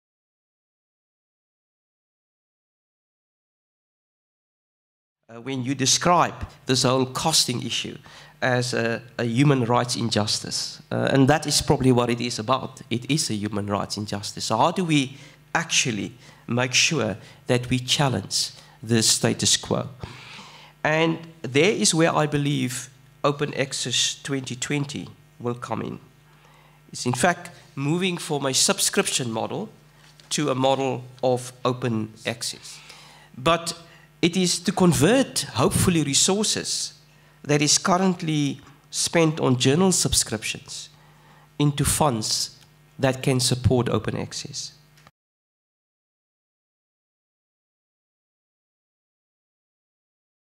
Prof Petersen on challenging the status quo in academic publishing
At the Open Science Scholarship Colloquium 2019 held at the UFS on 19 November, in collaboration with the Central University of Technology (CUT), Prof Francis Petersen, Rector and Vice-Chancellor of the UFS, commented on challenging the status quo in academic publishing and agreeing with Prof Ahmed Bawa (CEO: USAf) on Open Access being a human rights issue.